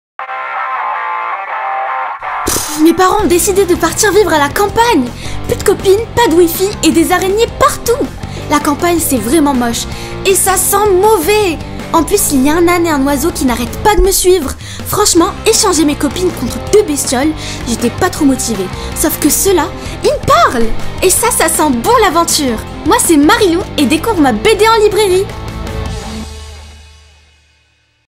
Marilou T1 - la bande annonce
Les parents de Marilou ont décidé de partir vivre à la campagne. Mais Marilou, la campagne, elle trouve ça plus que nul. Alors pas question de s'y plaire et encore moins d'y rester ! À moins que...
Découvrez Marilou, petite fille drôle et pleine de caractère, qui vient d'arriver à la campagne après un déménagement. Plus de copines, pas de Wifi, des araignées partout... Hors de question de rester là ! En plus, il y a aussi un âne, et un oiseau qui n'arrêtent pas de la suivre. En général, les animaux aussi, elle trouve ça nul, sauf que ceux-là... Ils parlent ! Et ça, ça sent bon l'aventure !